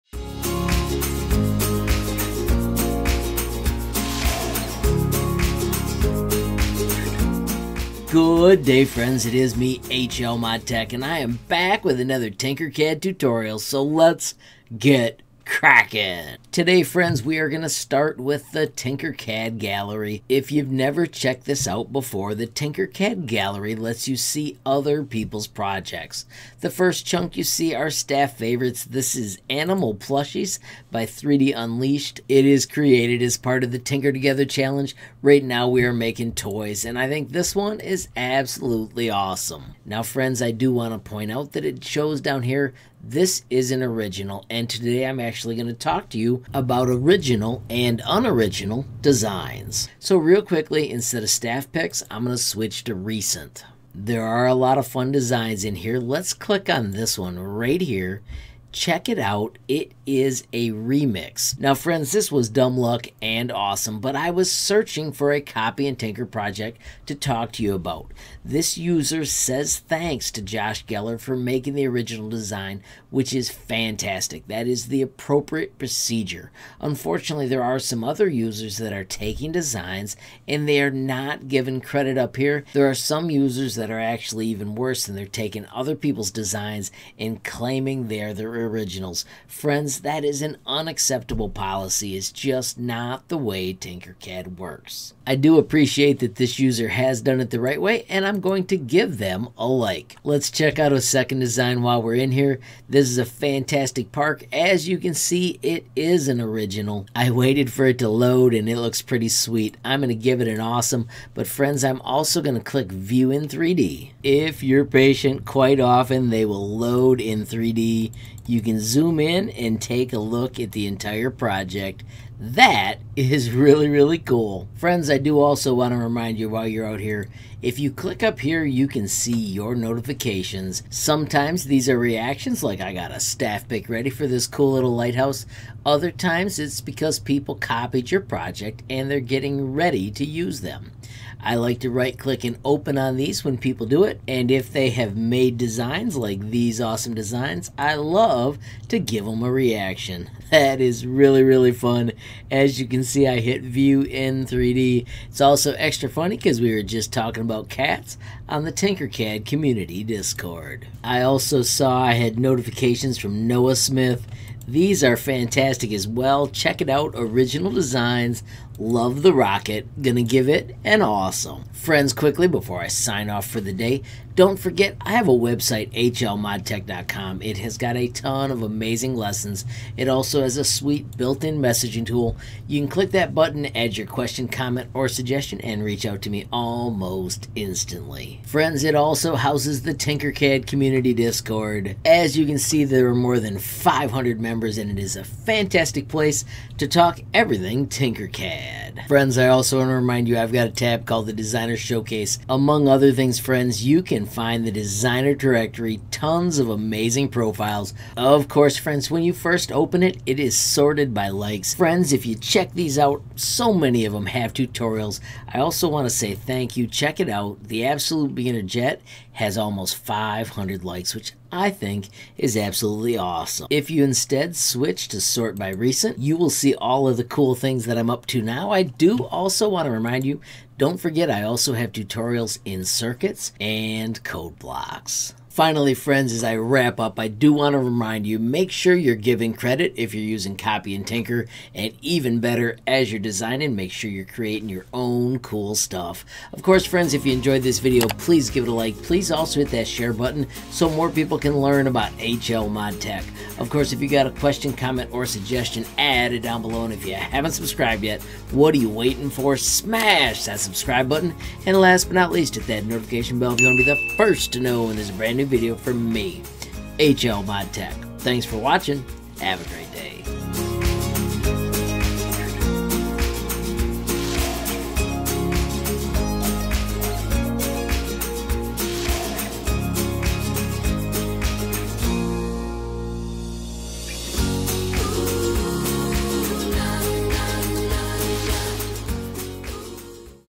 Proper Etiquette with the Tinkercad Copy & Tinker button
Today I highlight Proper Etiquette with the Tinkercad Copy & Tinker button. I believe Tinkercad Reactions ROCK! Copying work as your own does not. Just a quick reminder that taking Tinkercad projects and copying them into a new project does not make them your own. Be nice, have fun and Keep Tinkering.